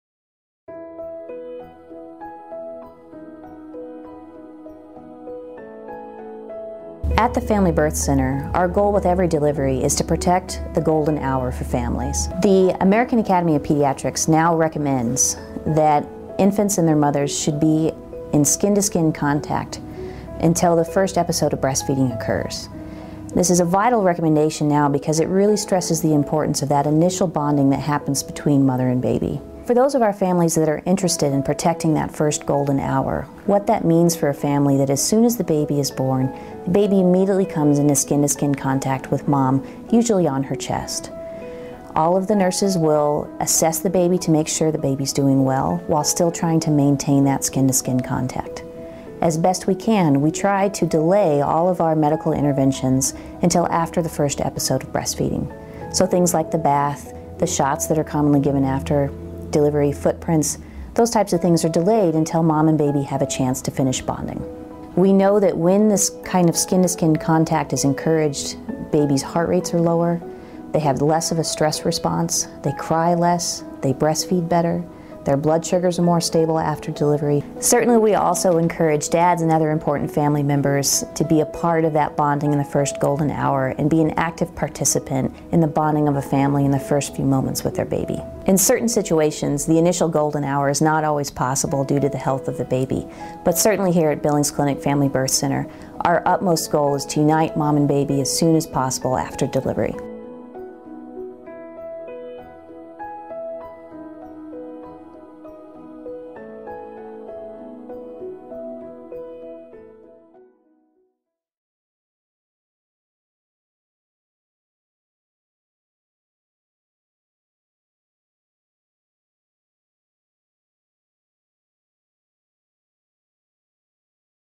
The Golden Hour | Billings Clinic
The Golden Hour is the special time for bonding between a baby and mom. Watch this video to learn the benefits of the Golden Hour and why we encourage skin to skin contact as soon as possible after birth at the Billings Clinic Family Birth Center.